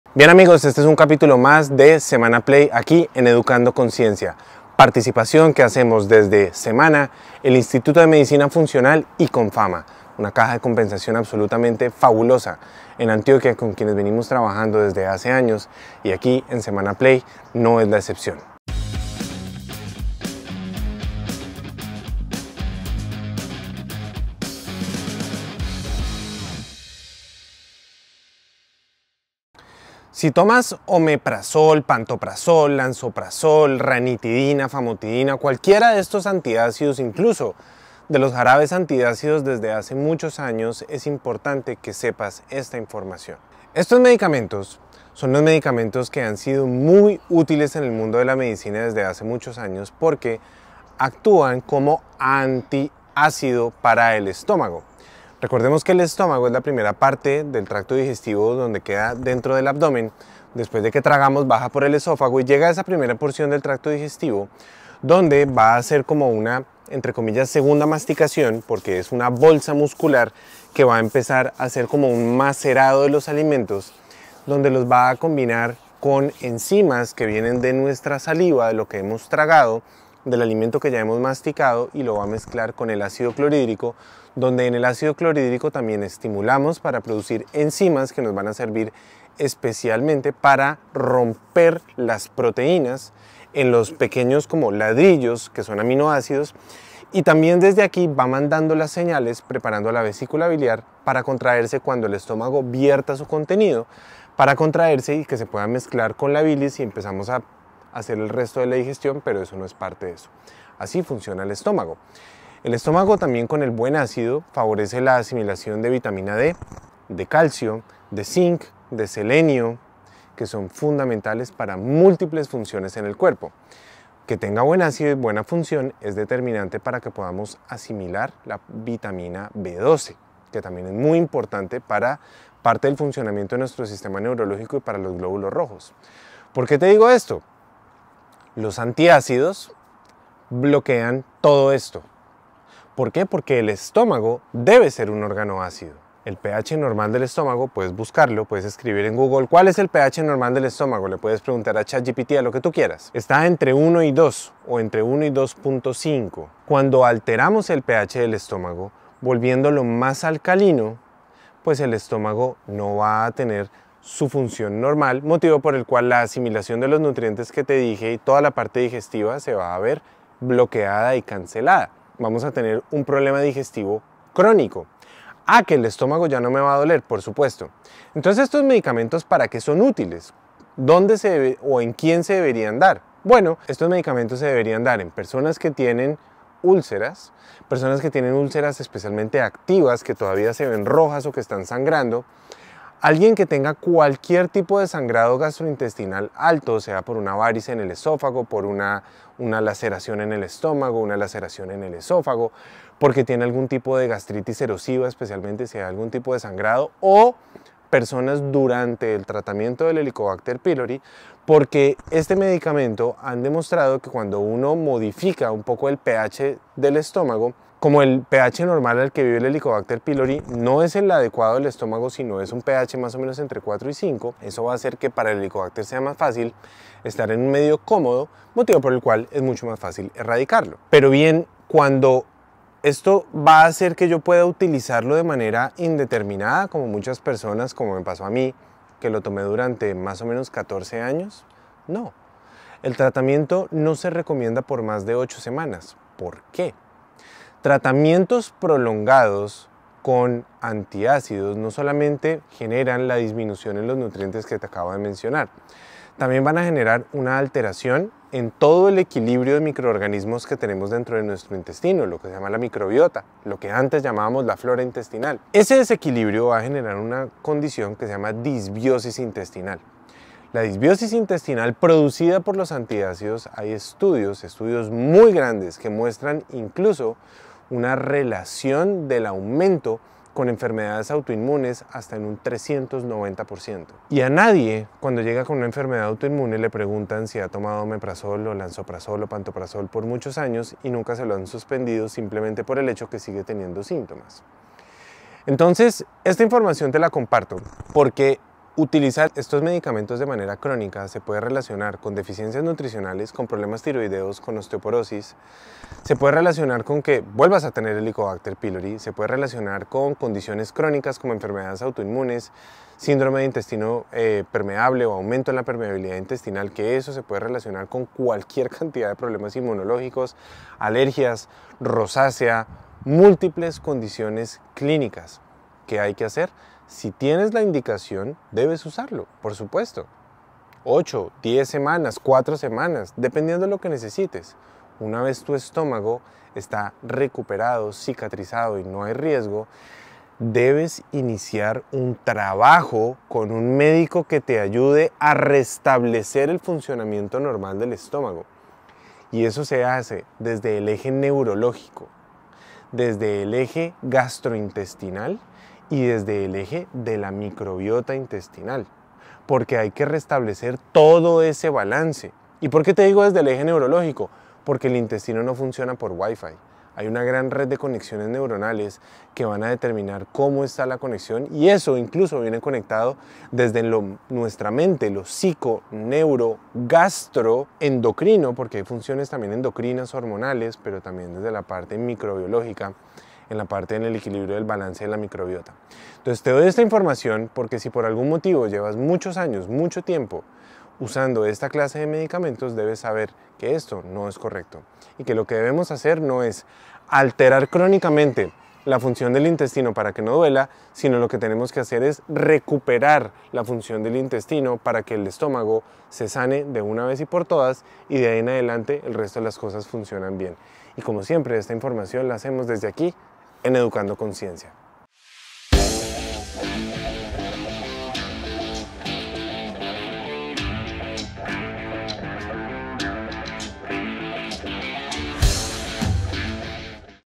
¿Por qué es peligroso tomar antiácidos por mucho tiempo?
En este episodio de Educando ConCiencia, con el apoyo de Comfama, el médico funcional Carlos Jaramillo habla de los peligros de tomar antiácidos de manera prolongada.